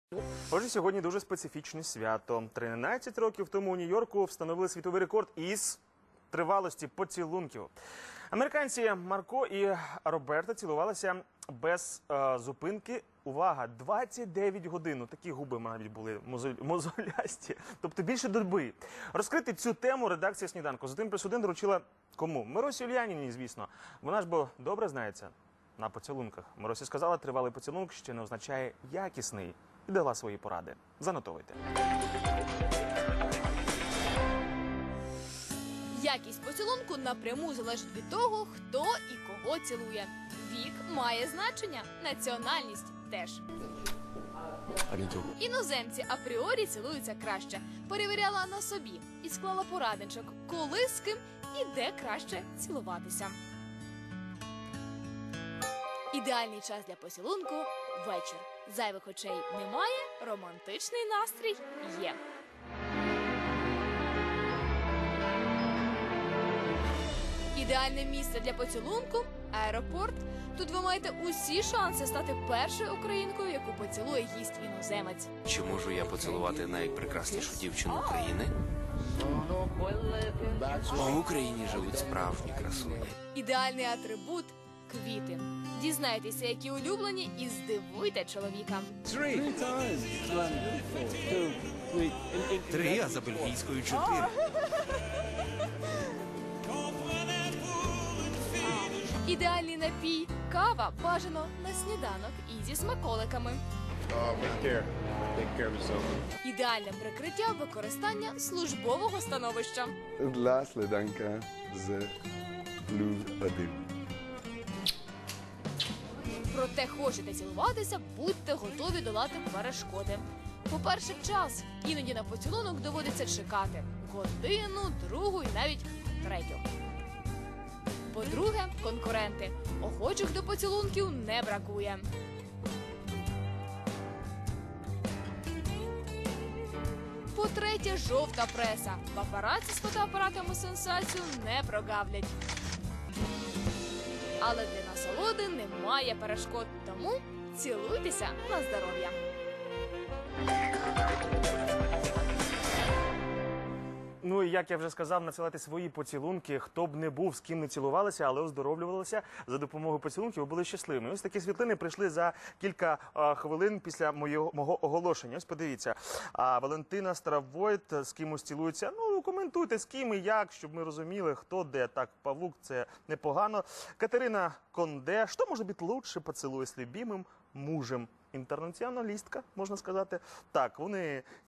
just a kiss
25 березня американська закохана пара потрапила у Книгу рекордів Гіннеса. За що? За найдовший поцілунок. Він тривав 29 годин. Але тривалий - ще не означає приємний. Поради від Мирослави Ульяніної